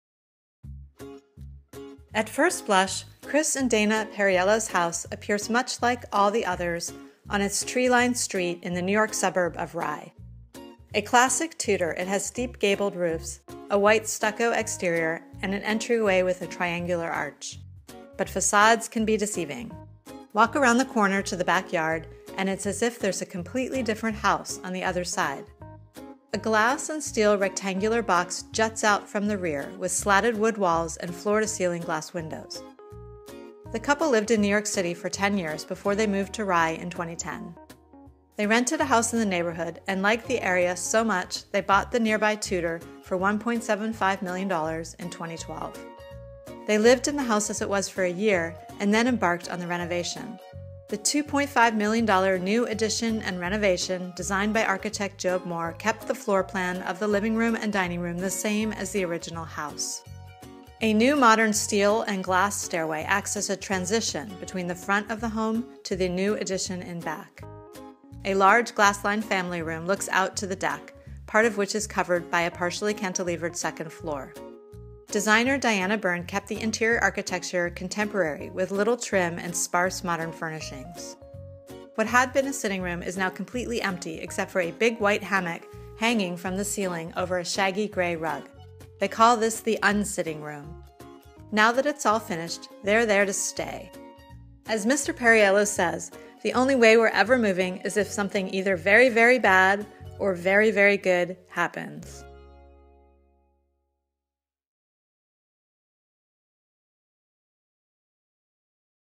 Classic Tudor Home With a Contemporary Twist
In Rye, N.Y., Chris and Dana Perriello's home includes a new addition in back with floor-to-ceiling glass windows, and a sitting room with a white hammock hanging from the ceiling. Photo: Dorothy Hong for The Wall Street Journal